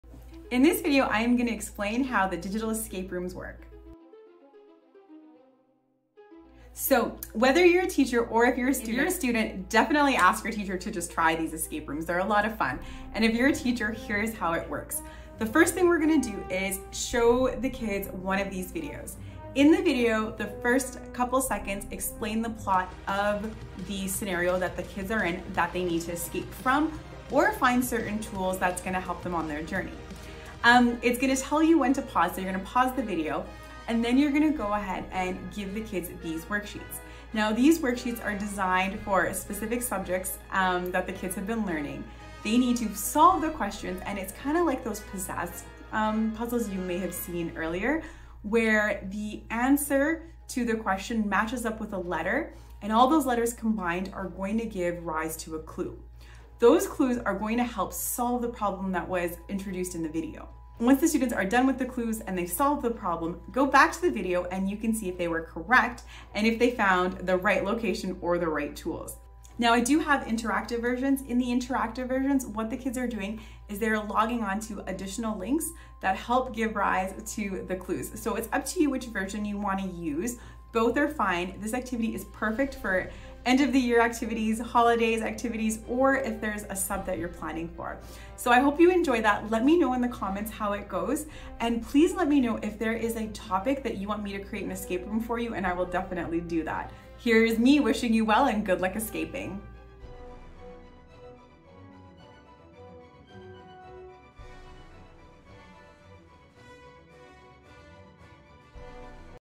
Math Escape Rooms
This is a fantastic review or introductory activity for specific concepts.

Teacher Instructions:
- Show students the video above and pause it when it says "Start Clue#1."
- You can have students work on the clues individually, or you can place them in groups.
-Each group member can work on an individual clue, or the whole group can work on a single clue.
- Solve the questions, find the letter above, and place the letter in the box to reveal the clue.
- Each clue will reveal a tool or get you close to a location.
- Feel free to start a timer to keep students on task.
- An answer key is included!